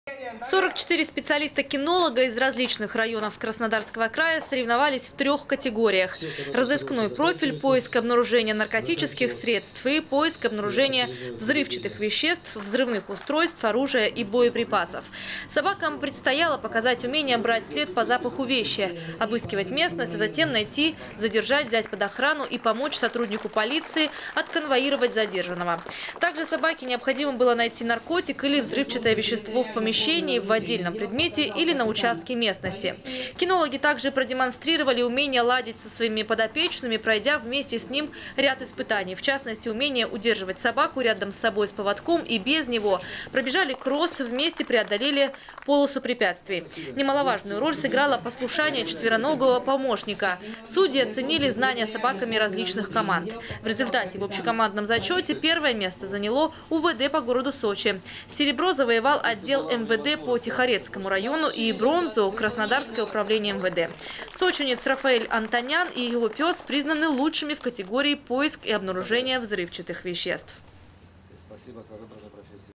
Сочинские кинологи- лучшие в крае. Новости Сочи Эфкате
В Сочи завершились краевые соревнования специалистов-кинологов. В них участвовали сотрудники территориальных органов МВД России. Состязания были приурочены к празднованию 106 годовщины создания кинологической службы в системе МВД.
Сочи. Новости.
Телеканал Эфкате.